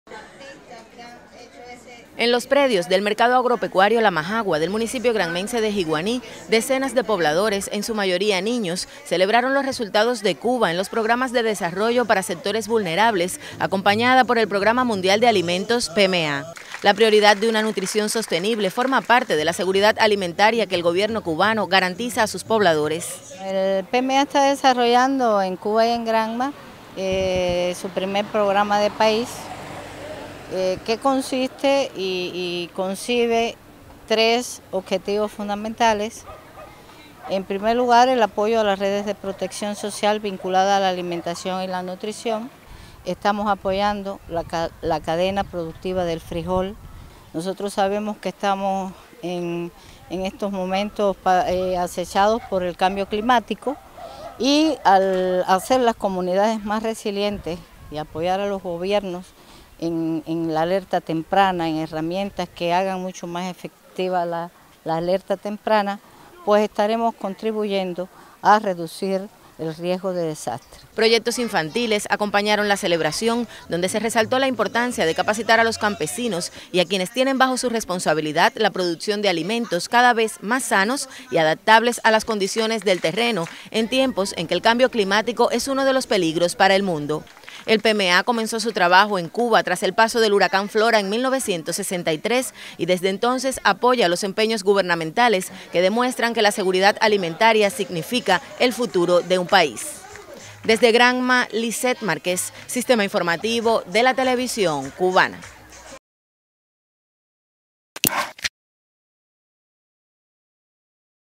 Conmemora Granma Día Mundial de la Alimentación
En los predios del mercado agropecuario La Majagua del municipio granmense de Jiguaní decenas de pobladores, en su mayoría niños, celebraron los resultados de Cuba en los programas de desarrollo para sectores vulnerables, acompañada por el Programa Mundial de Alimentos, PMA.